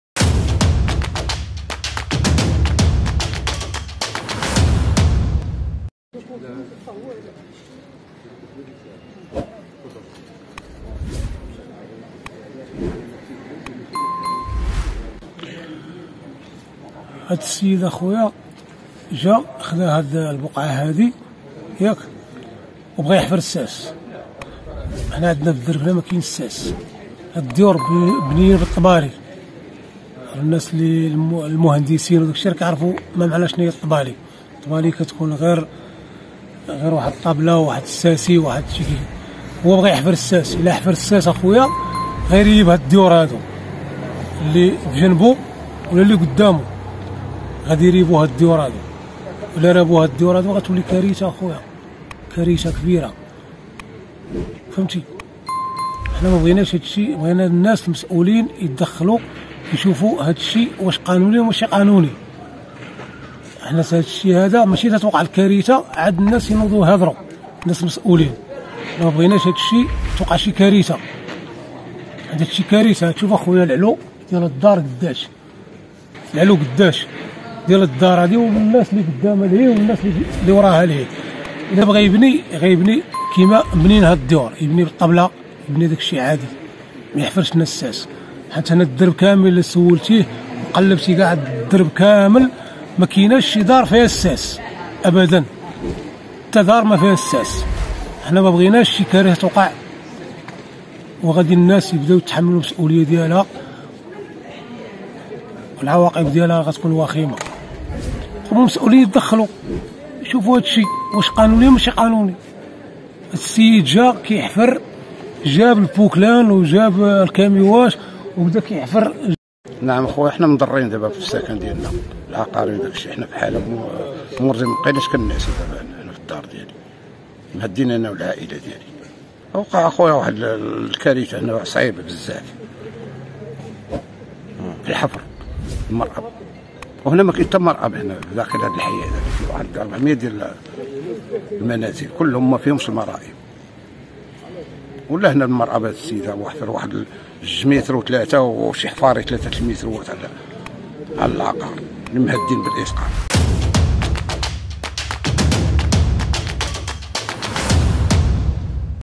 حفر ساس مرأب بالحي المحمدي ينذر بكارثة حسب شهادات الجيران
أخبار المغرب
# أخبار عاجلة